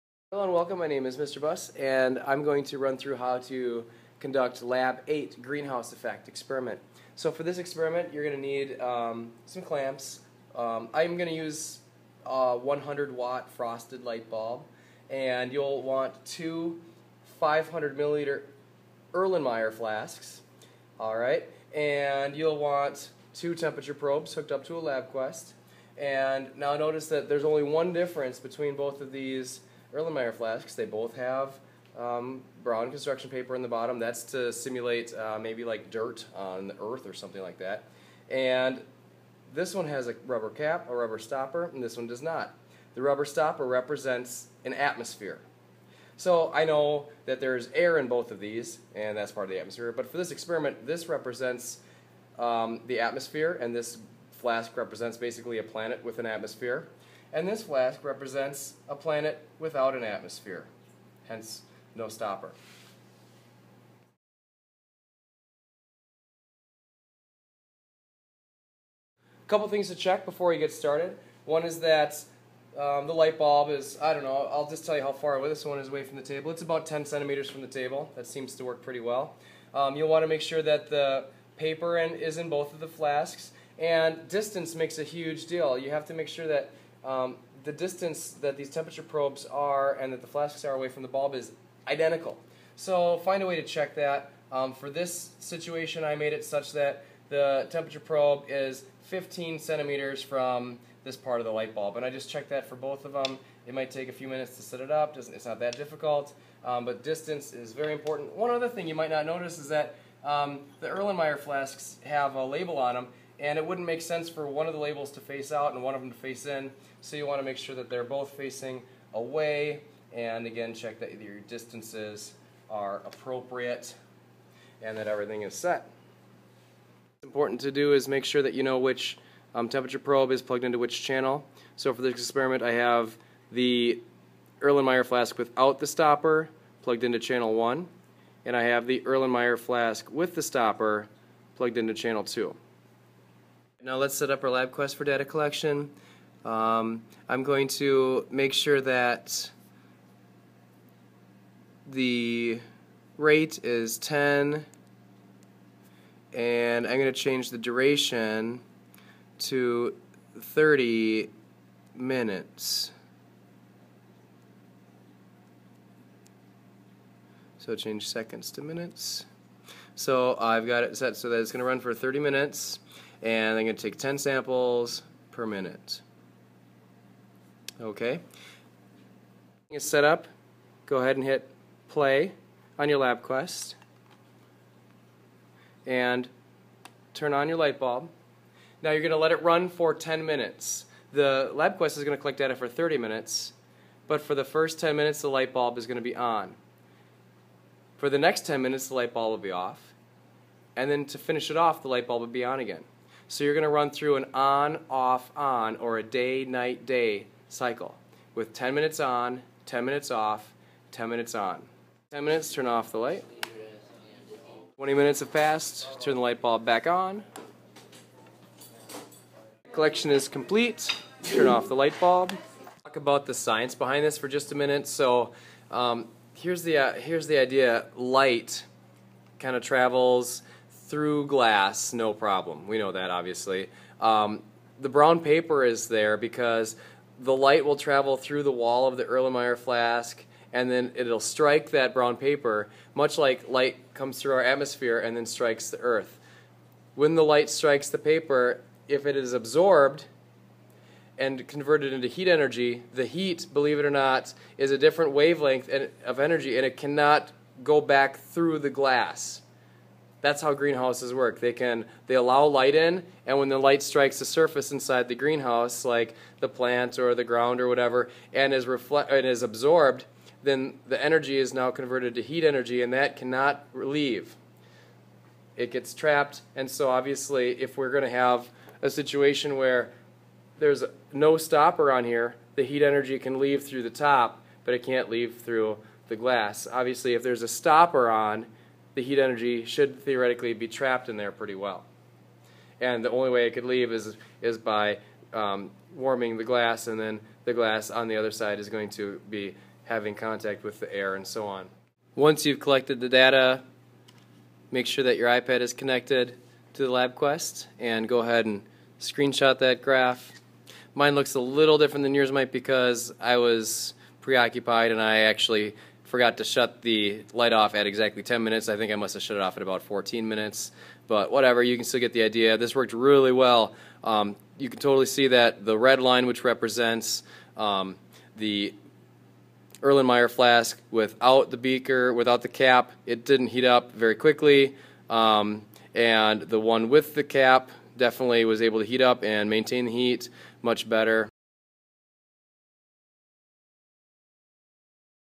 Lab 8 Greenhouse Effect
This is Mr. Buss Earth Science Lab 8:  Greenhouse Effect Explained